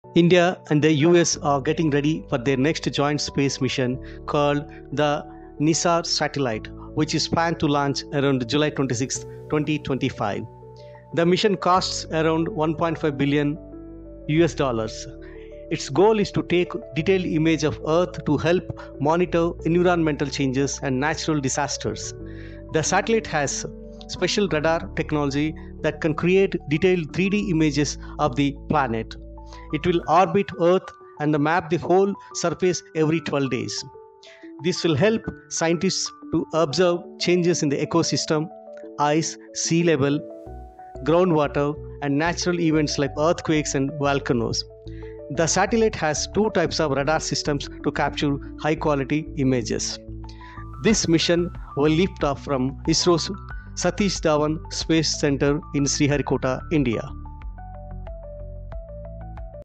India and US Prepare for Next Joint Space Mission with $1.5 b NISAR Launch Likely on July 26, 2025
India and the US are getting ready for their next joint space mission, called the NISAR satellite, which is planned to launch around July 26, 2025. The mission costs about $1.5 billion. Its goal is to take detailed images of Earth to help monitor environmental changes and natural disasters.

The satellite has special radar technology that can create detailed 3D images of the planet. It will orbit Earth and map the whole surface every 12 days.  This will help scientists observe changes in ecosystems, ice, sea levels, groundwater, and natural events like earthquakes and volcanoes.
The satellite has two types of radar systems to capture high-quality images. The mission will lift off from ISRO’s Satish Dhawan Space Centre in Sriharikota, India.